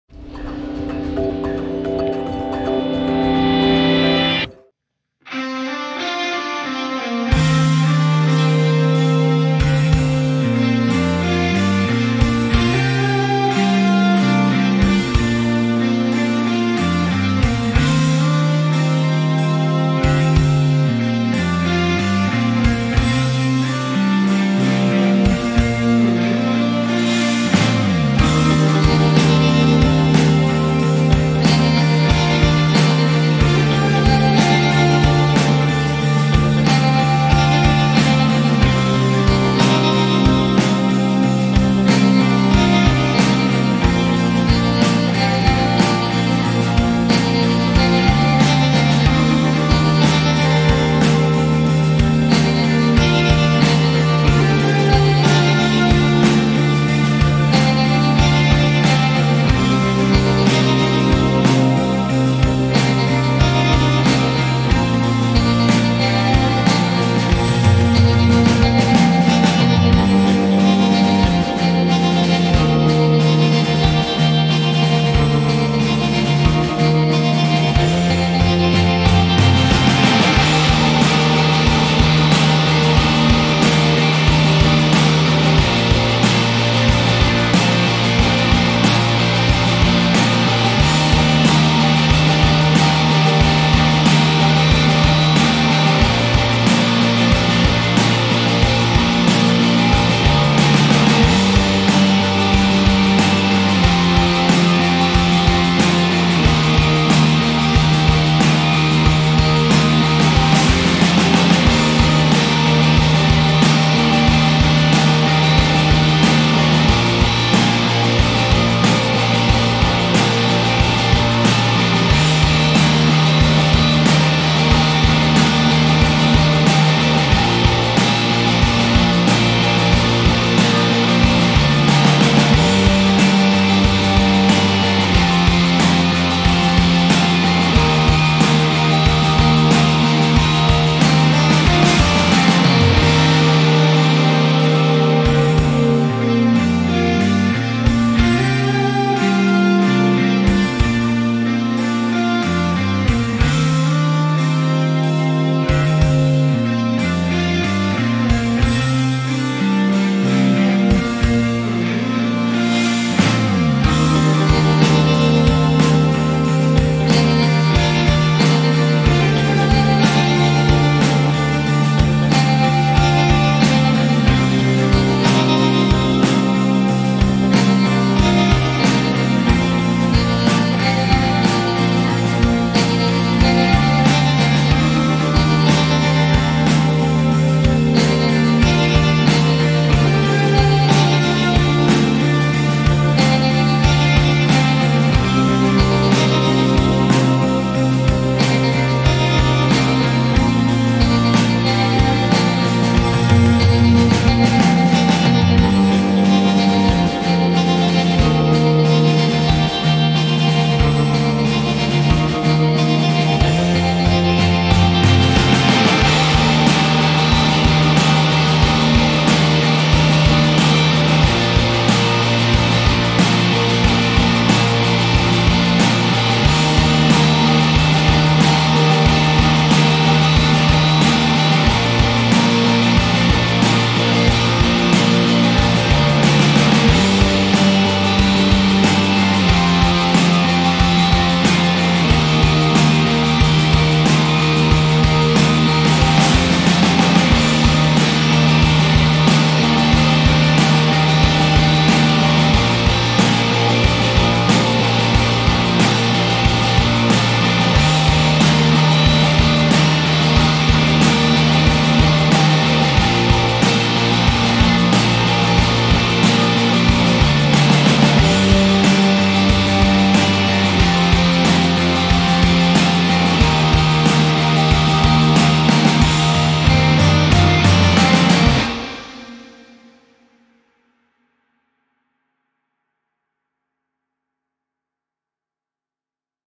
Shoegaze/Dream Pop Backing Track in B♭ major
Trap/Chill/Ambient/House etc... with Guitar track

Guitar:Vox SCD-55
Bass:Musicman Sterling
Guitar amp simulator:strymon iridium
Bass amp simulator:BIAS FX

KEY=B♭ major
BPM=92

0:00 Opening∼Intro
0:27 Verse
1:09 Pre-Chorus
1:20 Chorus
2:22 Intro
2:43 Verse
3:25 Pre-Chorus
3:35 Chorus